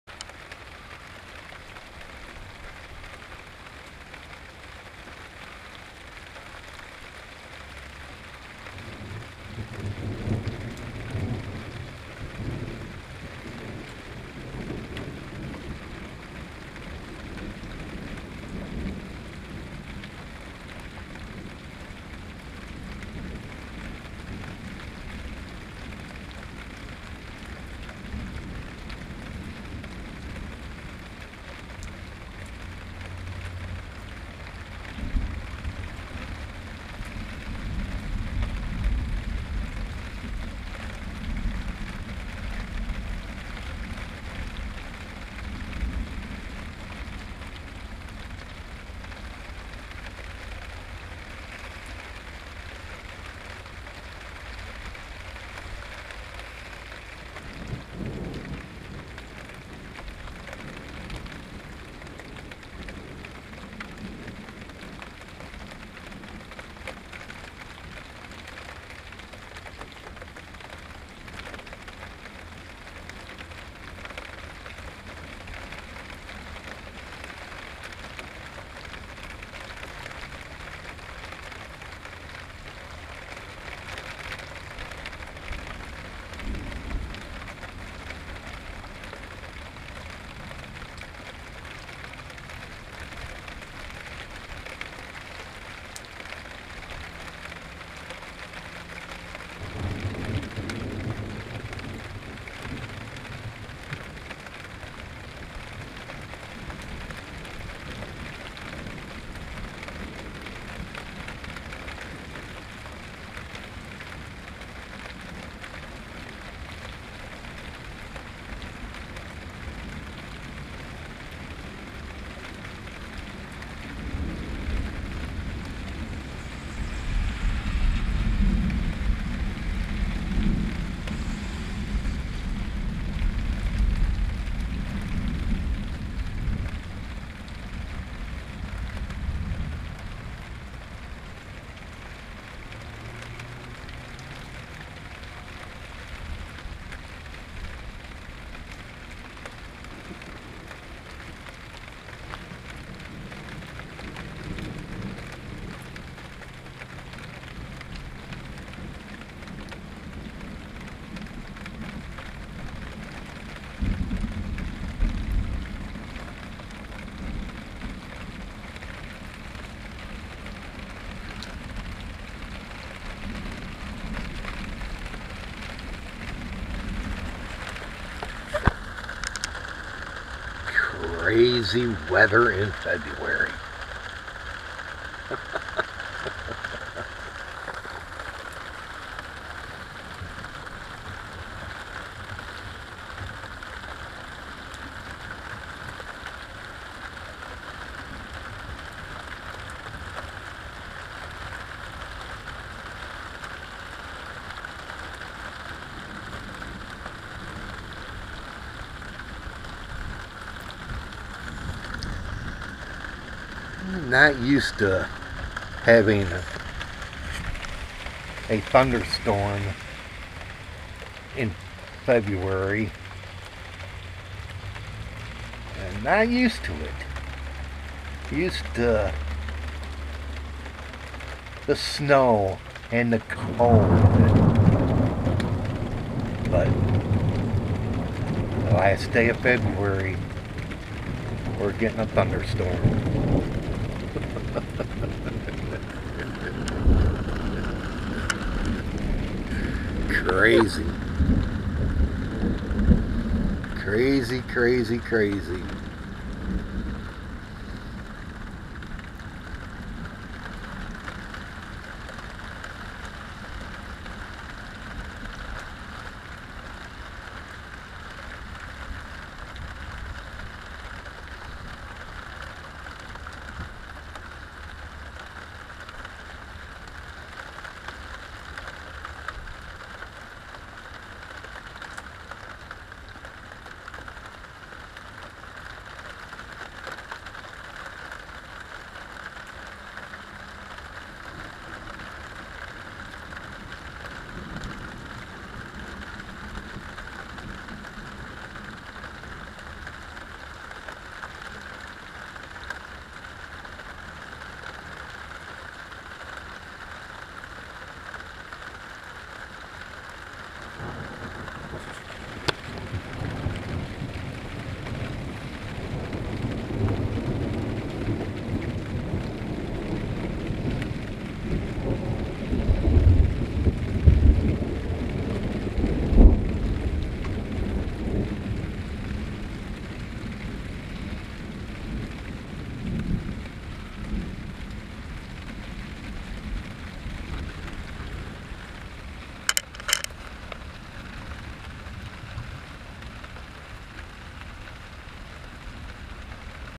THUNDERING AND LIGHTNING IN MICHIGAN 2/28/17
GETTING A THUNDER STORM IN FEBRUARY